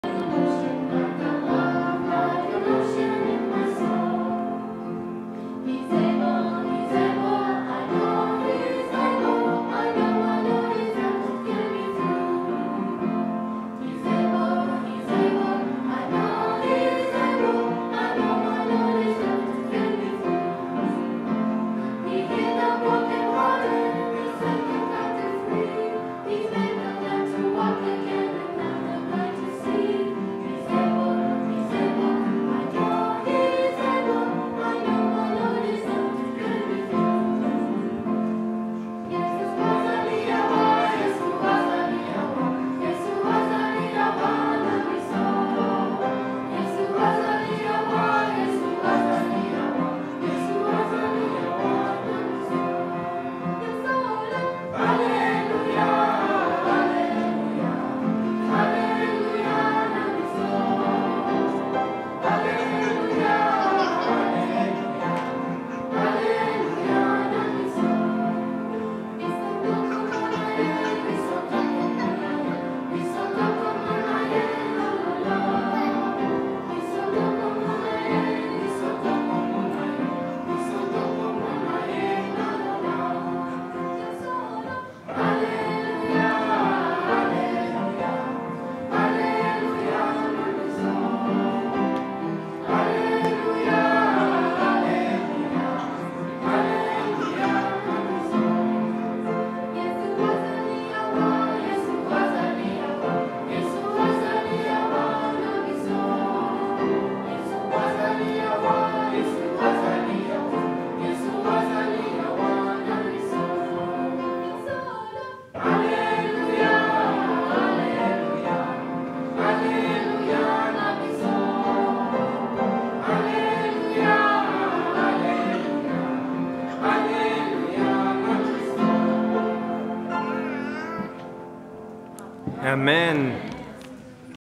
Glimpse of Shalom-by-the-Lake Youth Retreat
Just a short extract of a musical item that was performed by five youth visiting Ottawa church. They sung part of their song in Swahili.

Seventh Day Adventist Reform Movement - East Canadian Field Conference